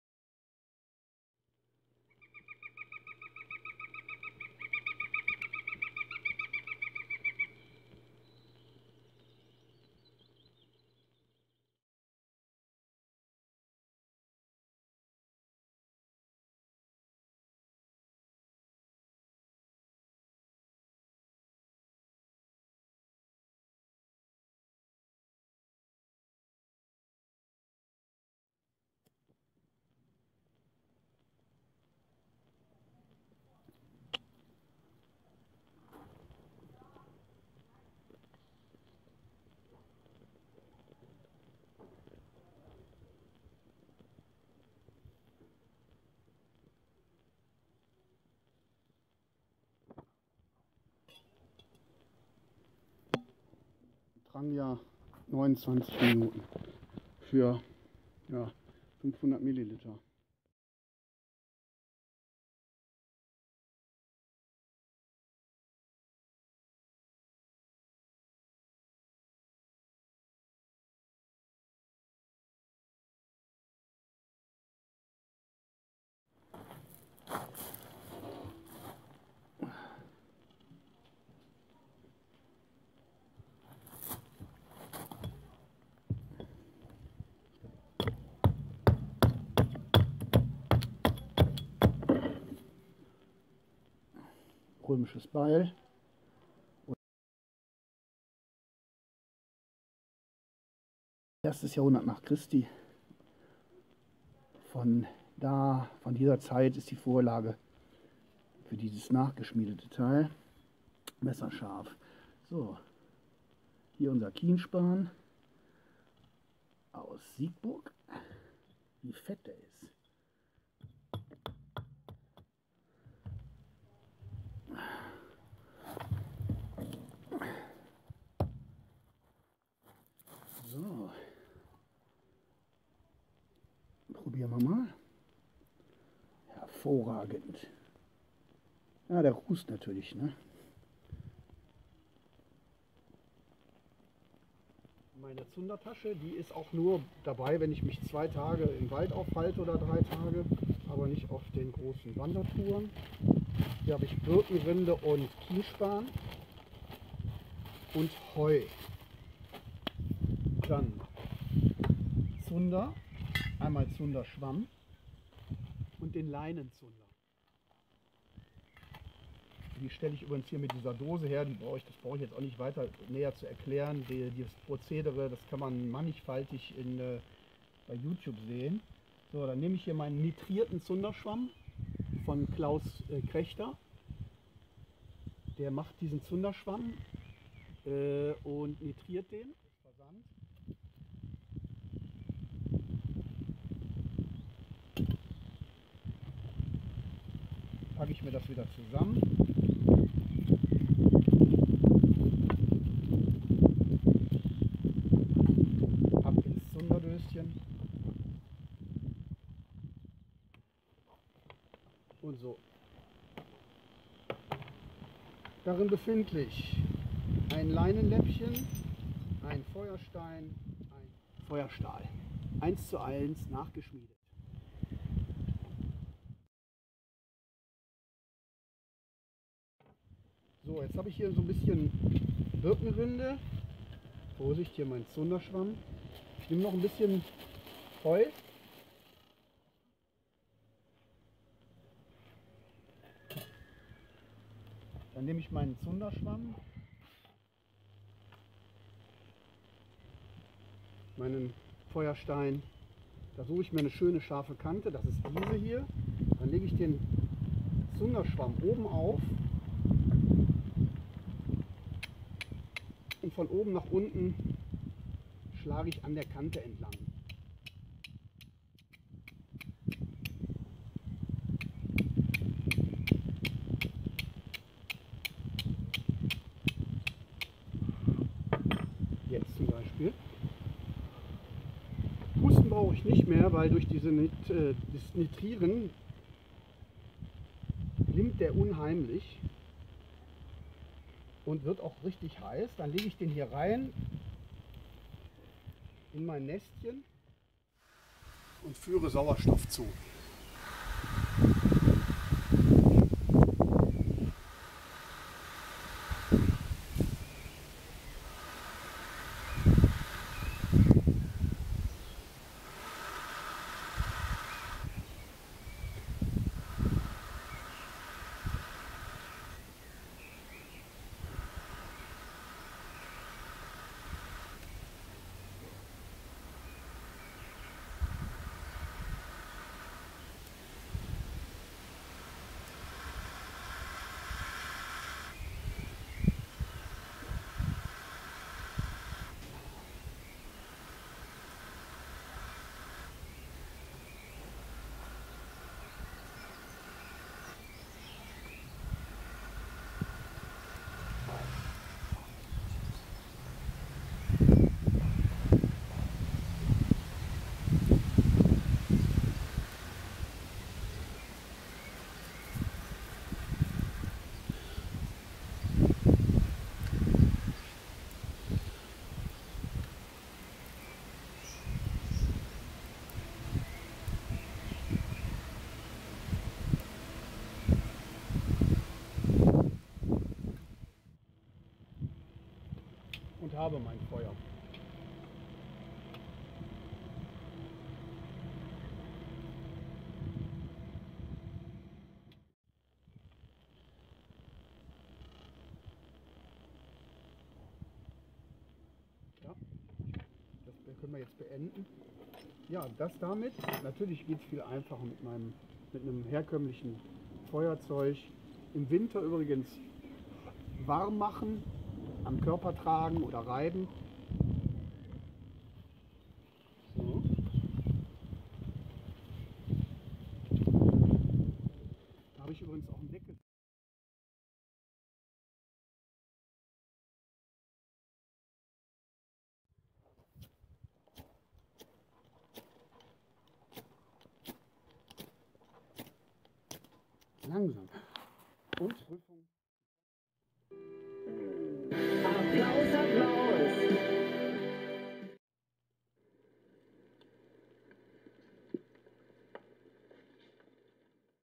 2018-Kochen und Feuer-01 Kocher & CO
Ein paar Informationen und Anwendungsbeispiele zum Thema Feuer & Co.